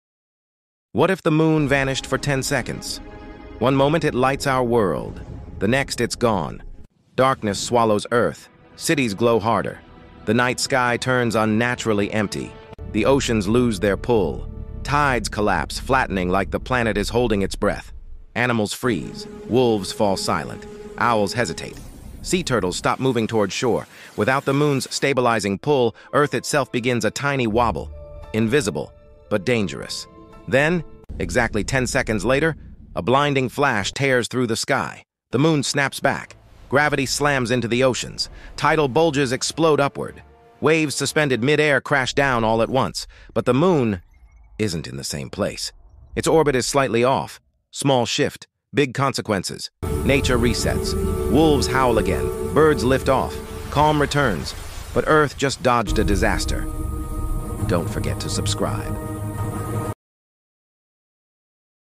WHAT IF… THE MOON VANISHED FOR 10 SECONDS?😱😱
What would happen if the Moon suddenly disappeared… even for just 10 seconds?
This short reveals the shocking chain reaction that would hit Earth instantly.
Darkness, collapsing tides, confused animals, unstable orbits — and a cosmic warning.

This is a hyper-realistic CGI explainer showing how deeply the Moon controls life on Earth.